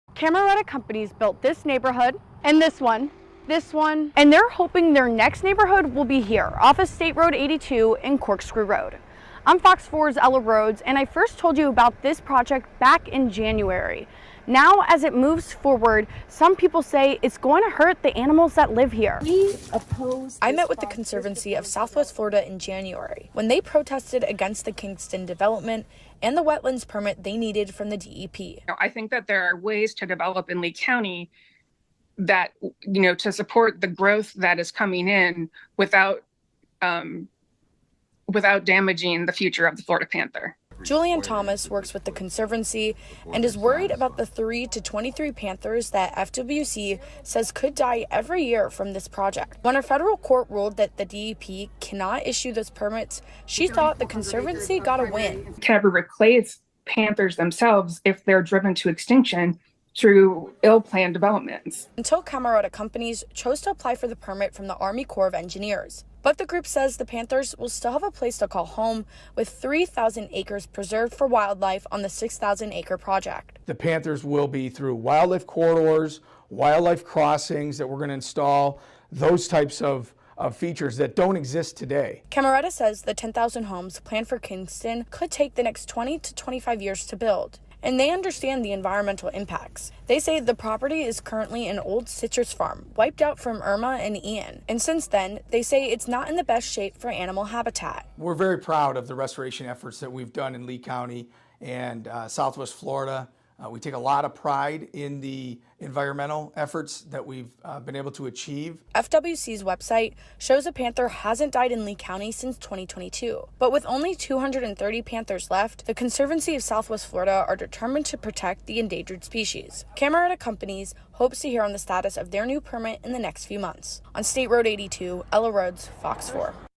CONSERVATION OR CONSTRUCTION? Kingston continues, with strong opposition
Environmental groups rallied against the Kingston development. When a federal court said the DEP can't issue the required permit, developers reapplied, but people are still worried for the panthers.